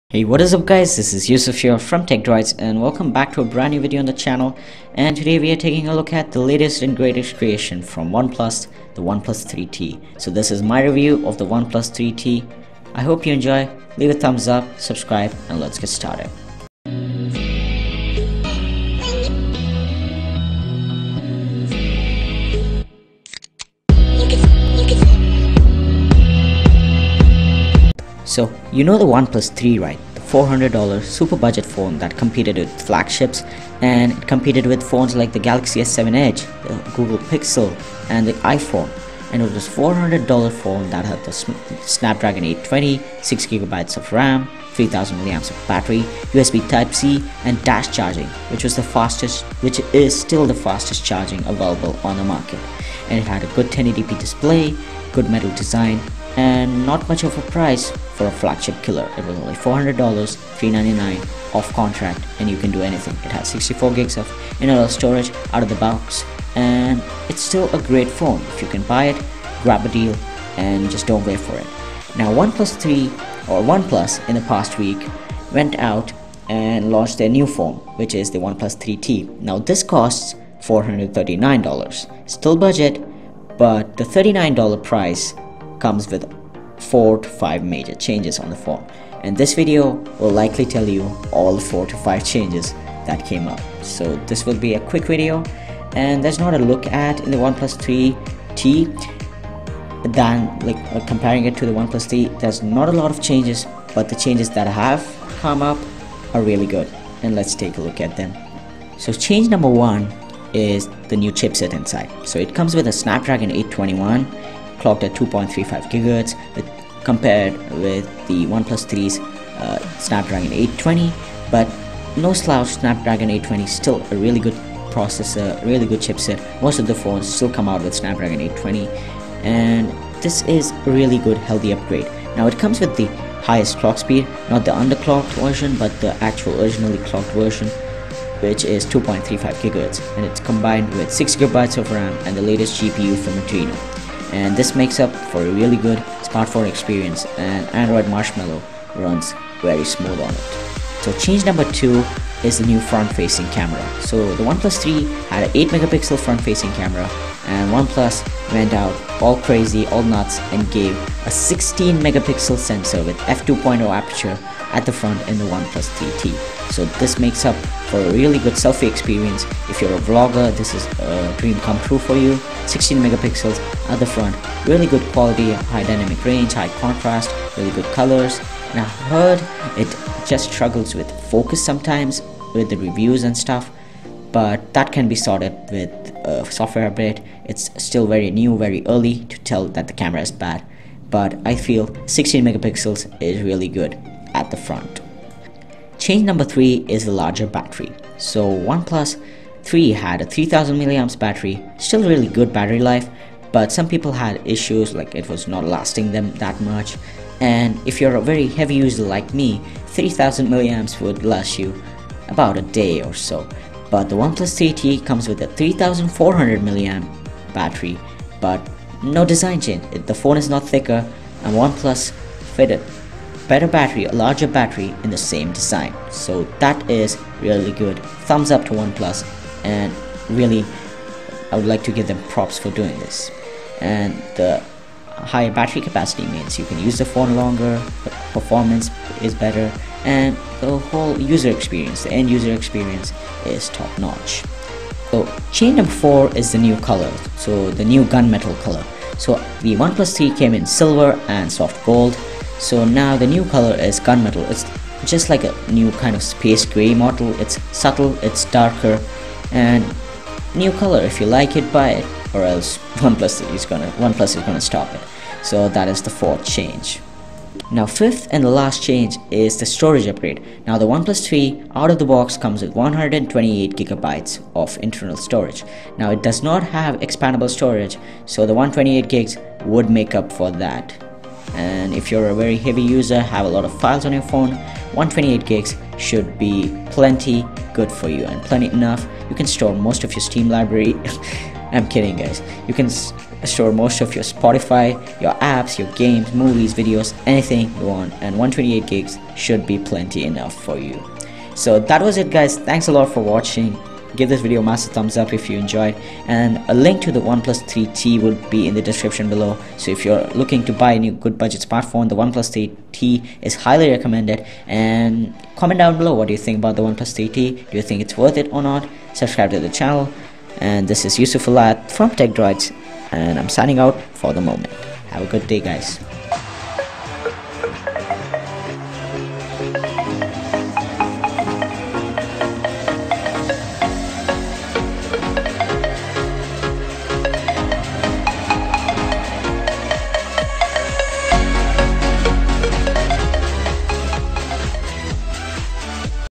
Oneplus 3T Review! I Best Budget Smartphone of 2016?
Oneplus 3T Review! Is this the Best Budget Smartphone of 2016?

and also leave feedback for future episodes and how I can improve the door to feedback is always open here at TECHDROIDS™